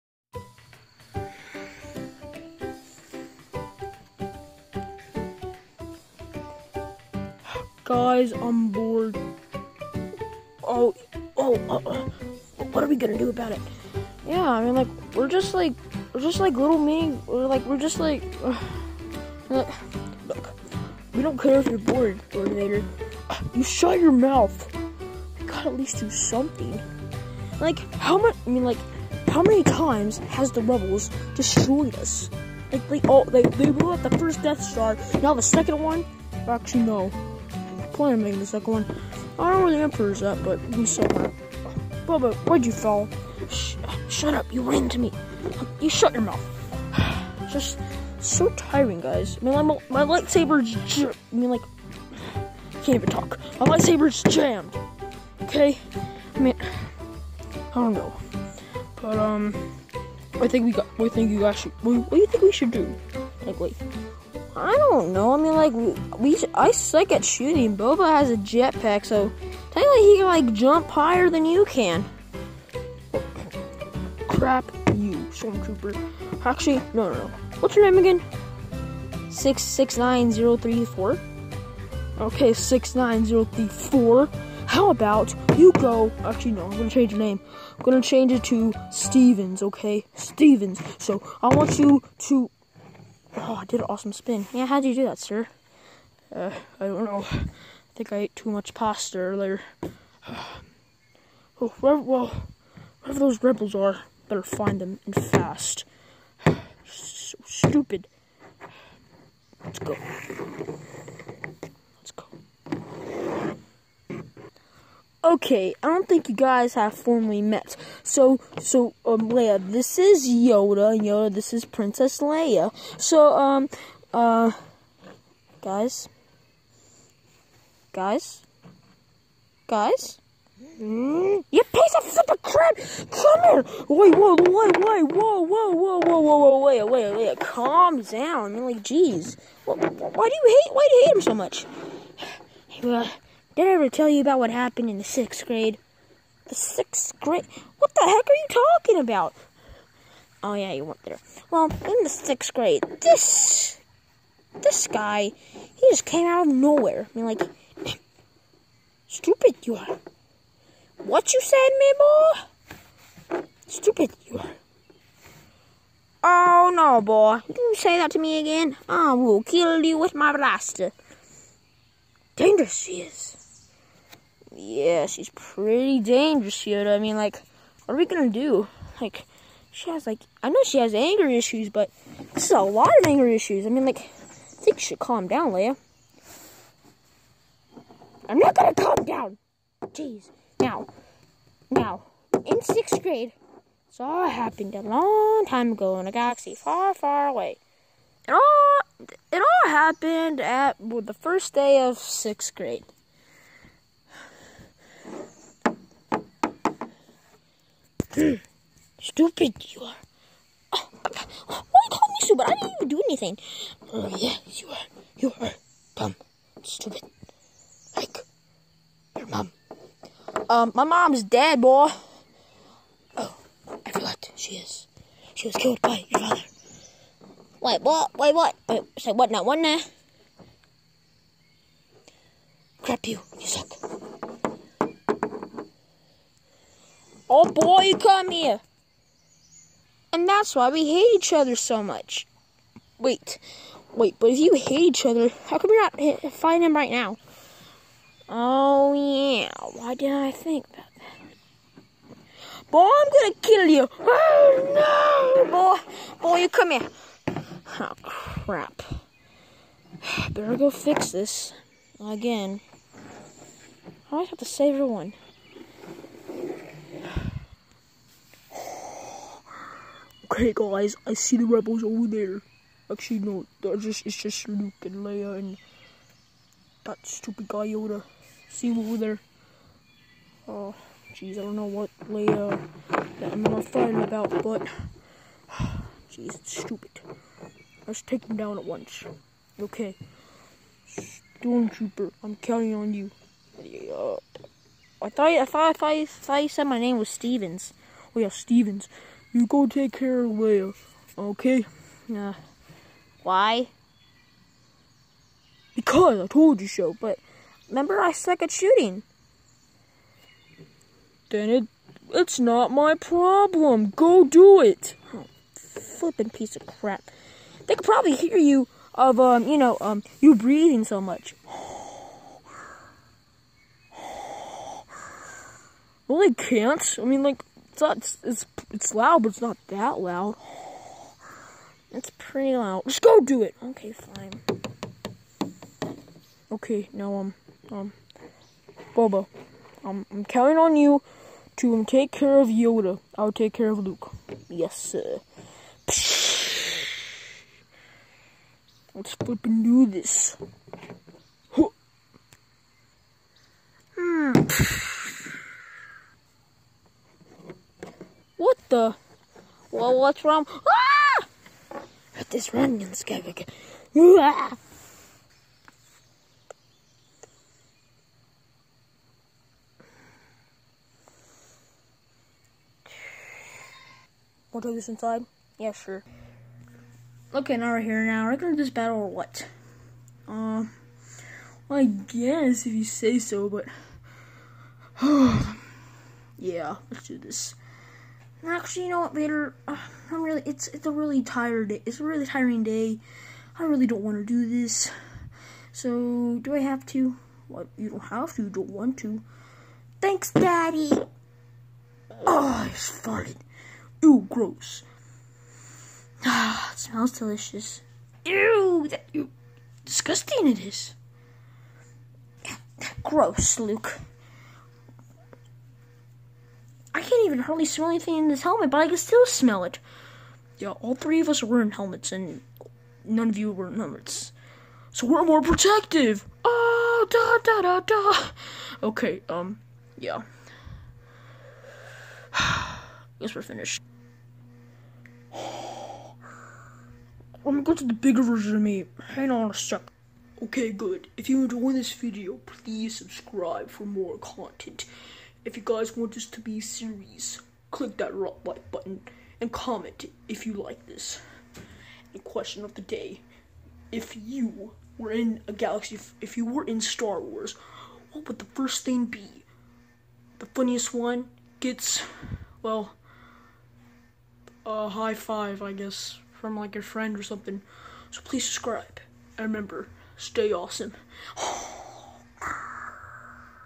456 Movie, Star Wars, The Story is Beginning
Darth Vader has nothing to do so he looks for Luke and his friends.